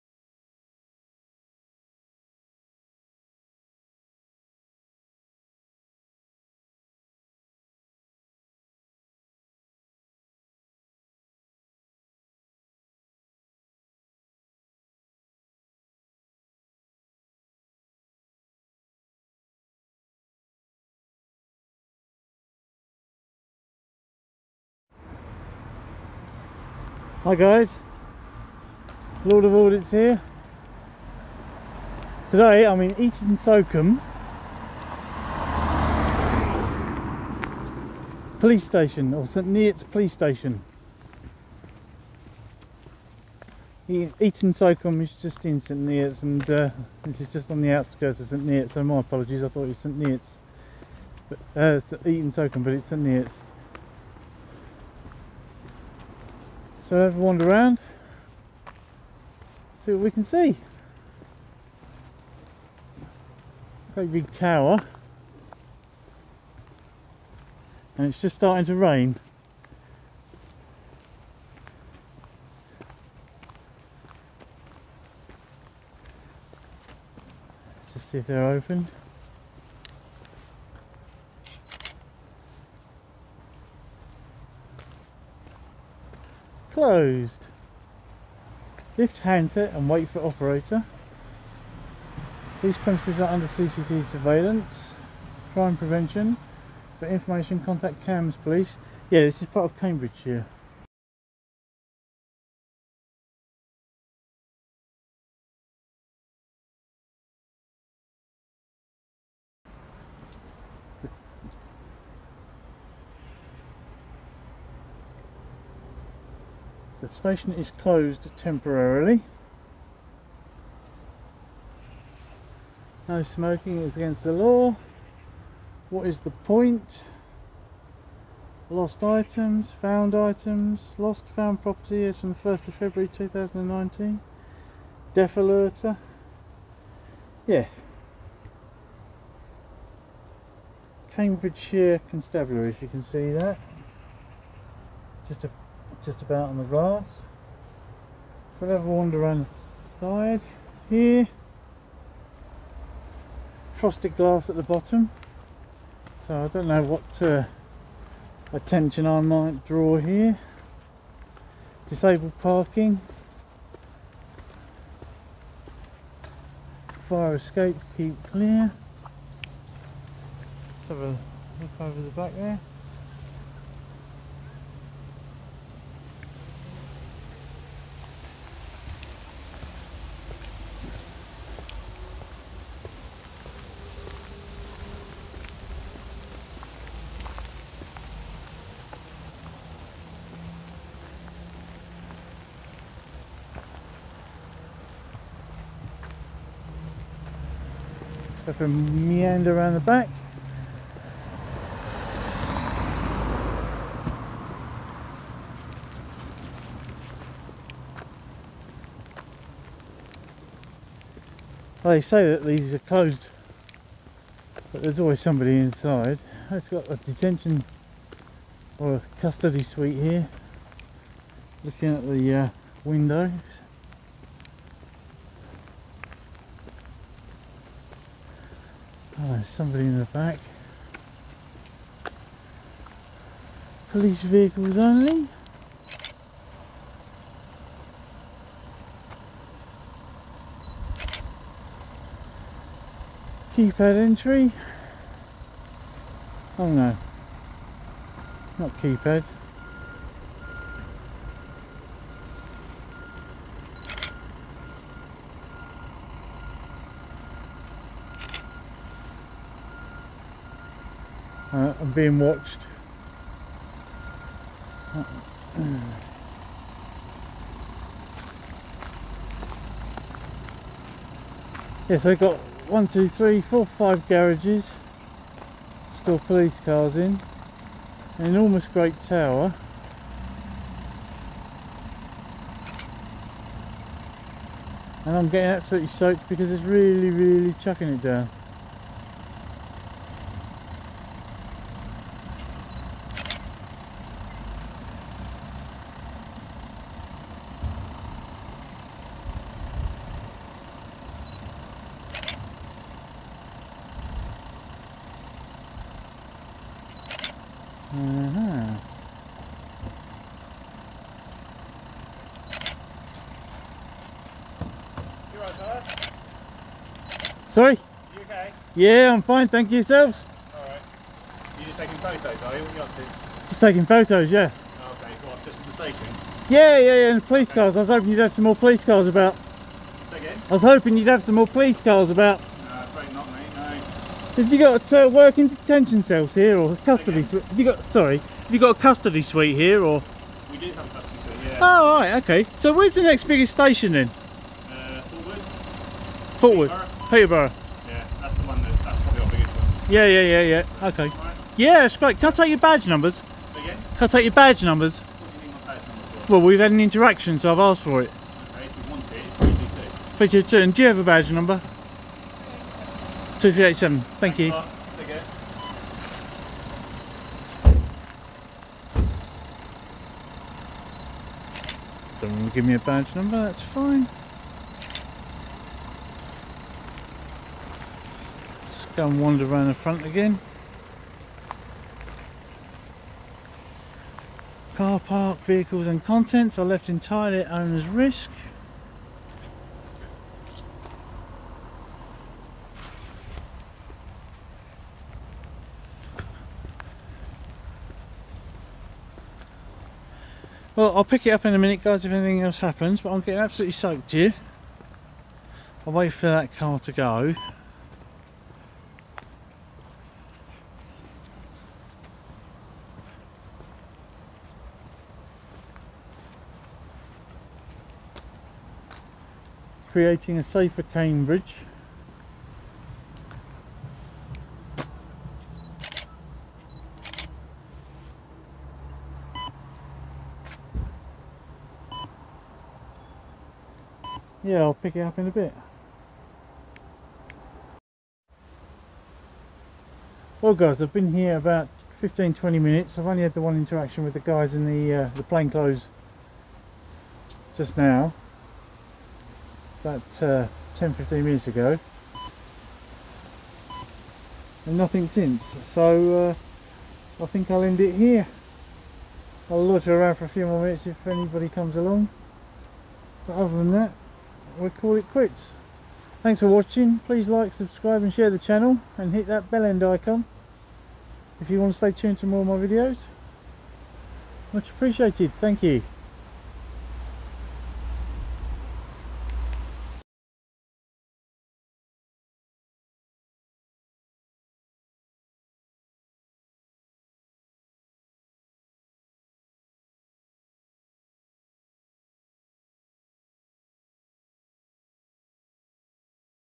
ST NEOTS POLICE STATION AUDIT - YOU CANT FILM HERE
Today, I visited St Neots Police Station in Cambridgeshire to carry out an audit, where I was told " you cant film here "